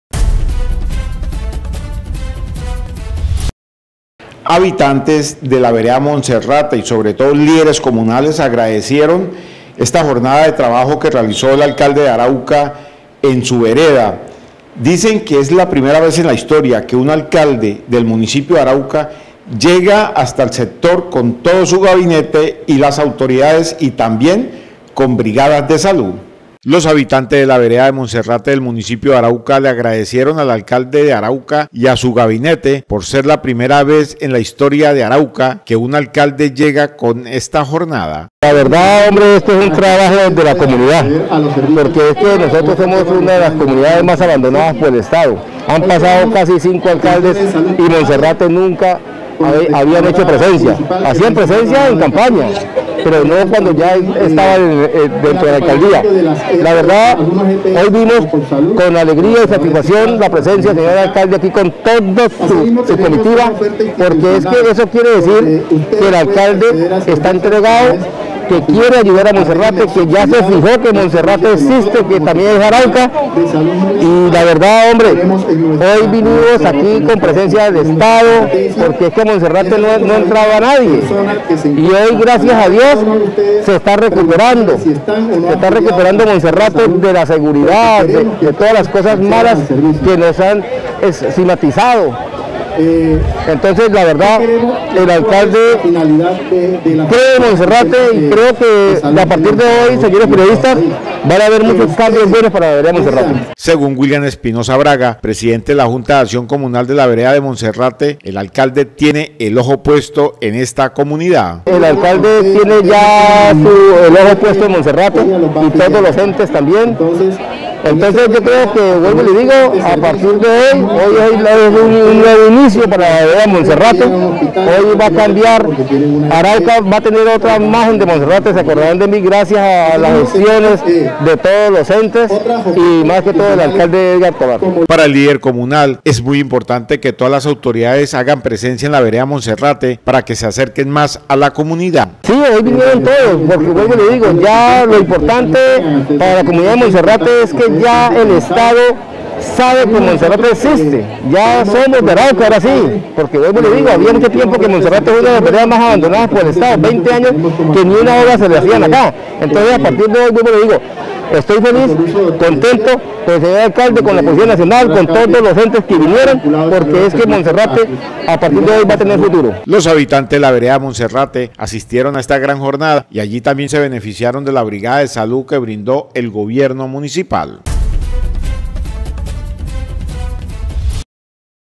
PRIMERA VEZ EN LA HISTORIA QUE UN ALCALDE LLEGA CON TODOS SUS SECRETARIOS A LA VEREDA MONSERRATE
PRIMERA VEZ EN LA HISTORIA QUE UN ALCALDE DE ARAUCA LLEGA CON TODOS SUS SECRETARIOS A LA VEREDA MONSERRATE